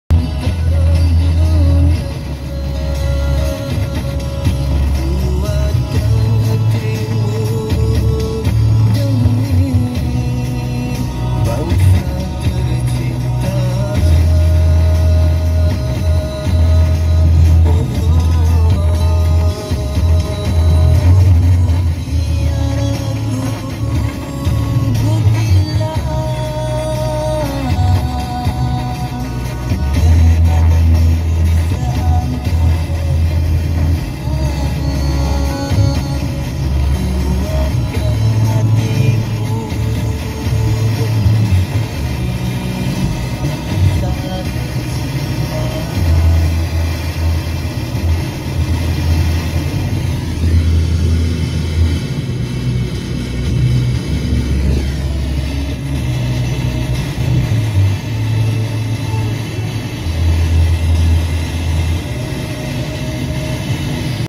Radio Thailand Yala FM 95.0MHz received in Selekoh, Perak, Malaysia (PART 2)
- Frequency: FM 95.0MHz
- Power: 1kW
- Transmission site: Khao Pok Yo, Bannang Sata District, Yala Province, Thailand
- Receiving site: Selekoh, Perak, Malaysia
- Distance: 276km
- Receiving date/time: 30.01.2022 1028H (UTC+8)
- Language: Thai & Pattani Malay
- Receiver: Clarion CY23G